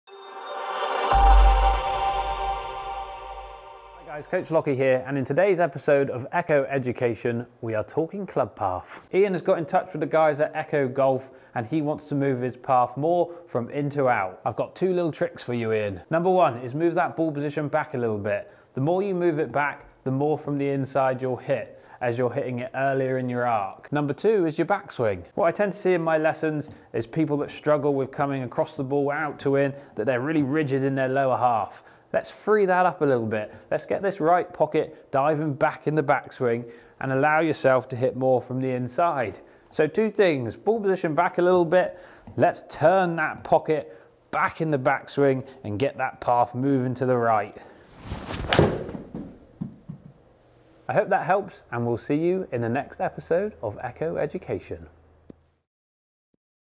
ECCO Education: Move Path In to Out with Coach Lockey
In this episode of ECCOEducation we are focusing on your club path. Ian messaged us to ask how he can move his path more from in to out, and Coach Lockey’s got the answers 👊 Check it out…ECCOEducation